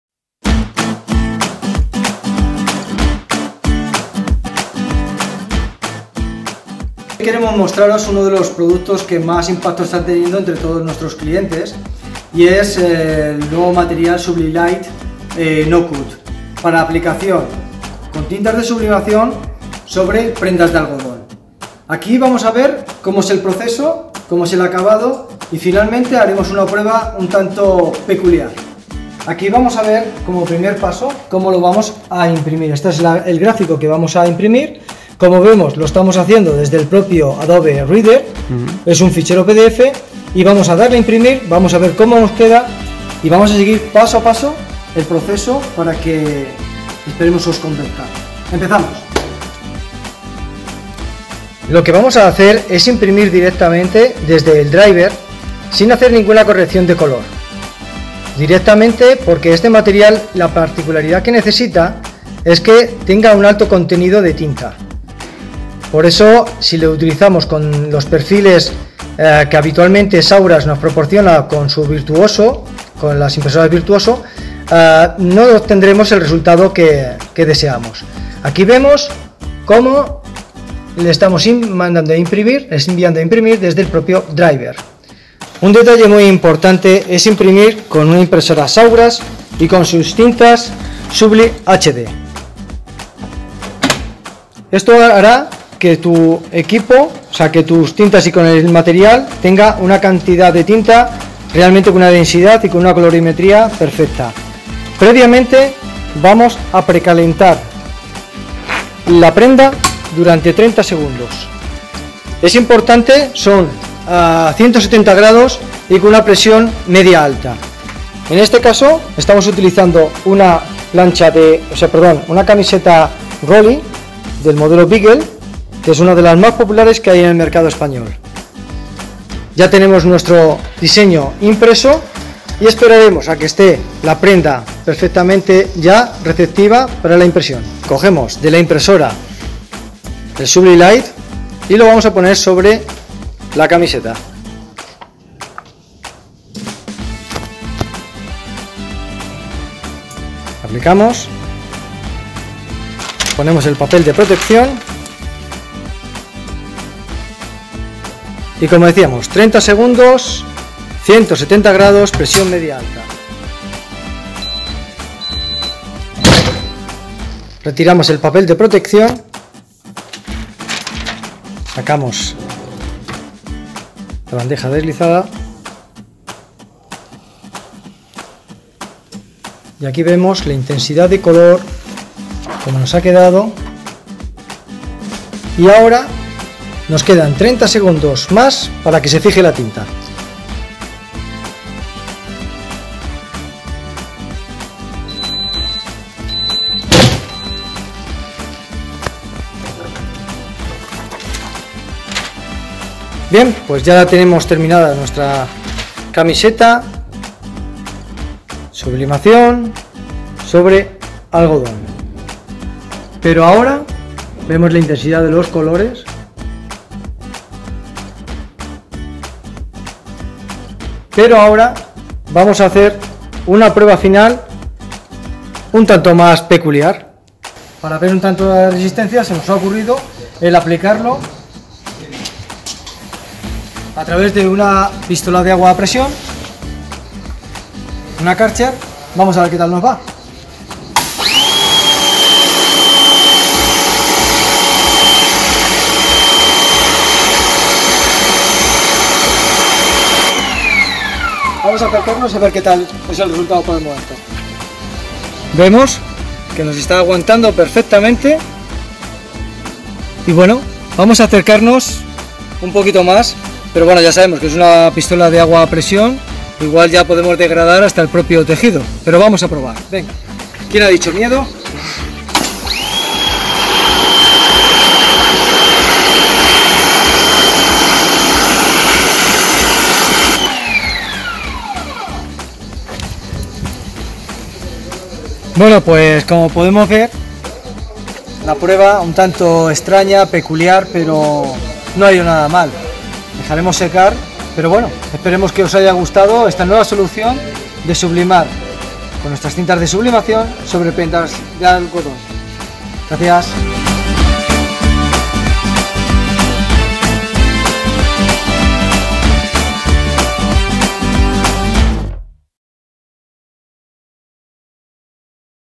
Forever Subli-Light (No CUT) - Proceso de uso y prueba final.
Os mostramos el proceso de creación con subli.light (no cut) y una posterior prueba peculiar sobre resistencia y aguante.
¡No os lo perdáis!